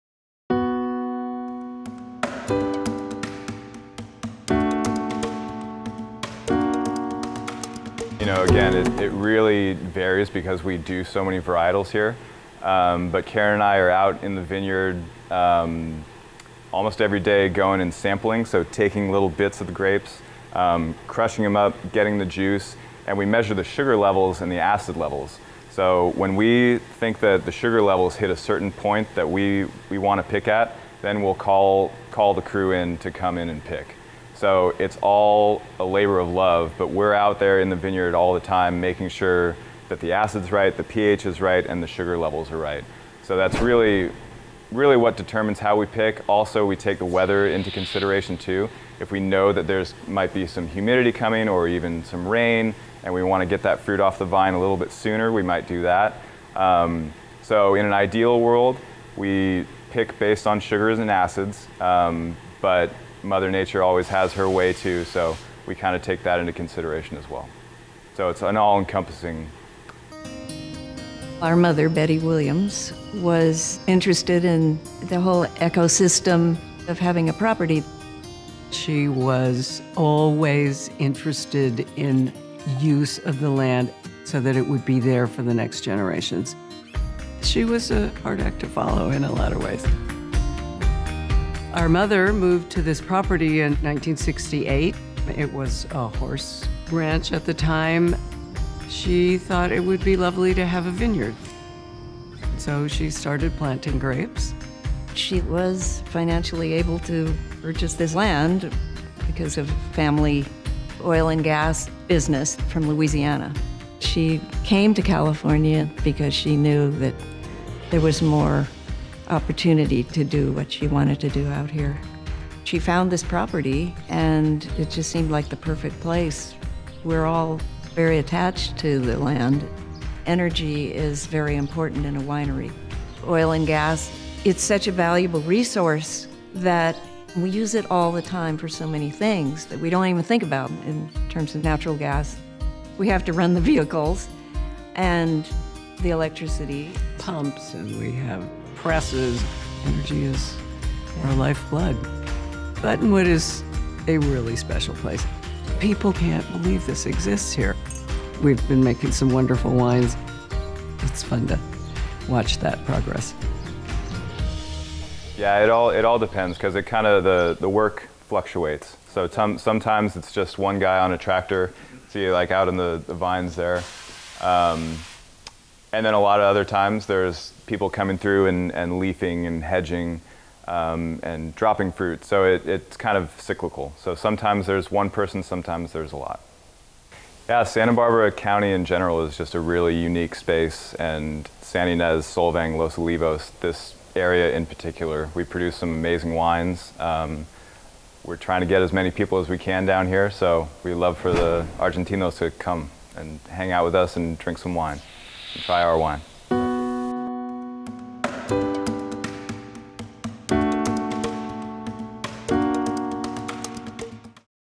Buttonwood Farm and winery 3 Parte
La granja ecuestre al norte de California hoy desarrolla grandes vinos en esta denominación. A lo largo de los años, la comprensión de lo que crece bien en la parte del Valle de Santa Ynez y los gustos por el vino han evolucionado y se ha injertado algunas de las vides originales en otras variedades. El mayor cambio es la incorporación de las variedades de Rhône Syrah, Grenache Noir, Grenache Blanc y Viognier.

Facebook  Aromas del Terruño